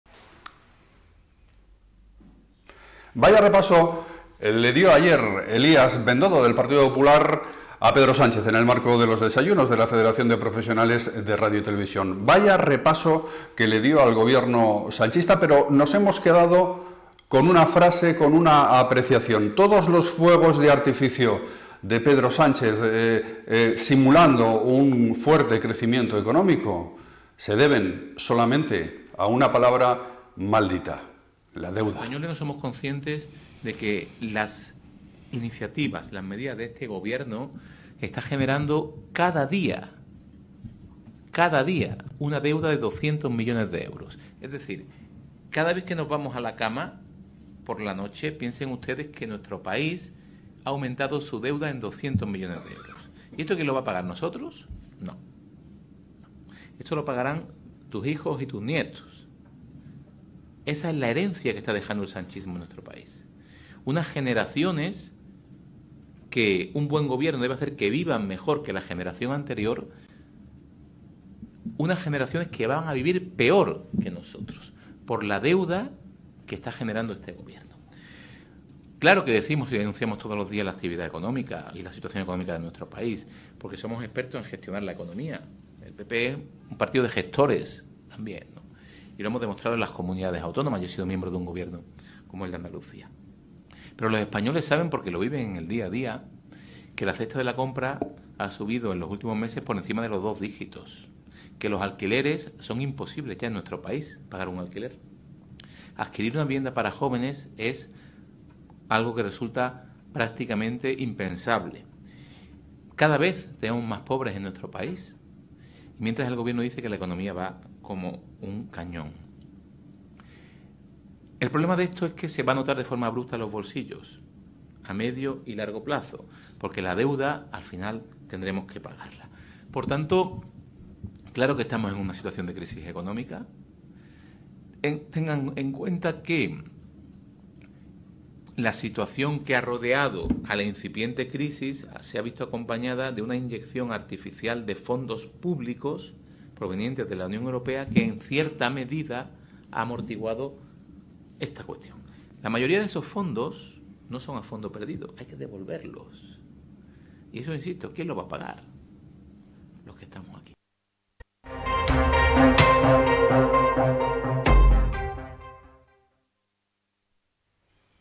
Bendodo deja en evidencia a Sánchez: desmonta con datos la "brutal deuda" de España
Elías Bendodo hace balance de la gran deuda que está dejando Pedro Sánchez y que afecta directamente a cada uno de los españoles.

Infórmate de las últimas noticias de política, economía y corazón de España y del mundo